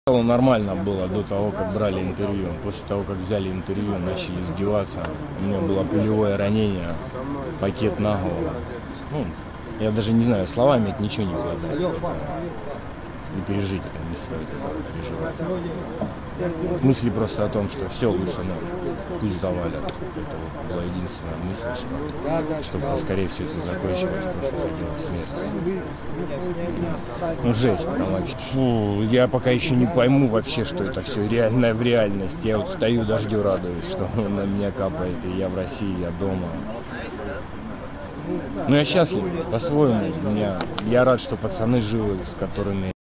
Звільнена з полону русня скаржаться на камеру про "страшні тортури"! БРЕХНЯ ВІД РУZКІХ! Новини LIVE!
Звільнена з полону русня скаржаться на камеру про "страшні тортури"

Зверніть увагу на повністю здоровий зовнішній вигляд орків. Навідміну від наших змучених знущаннями бійців.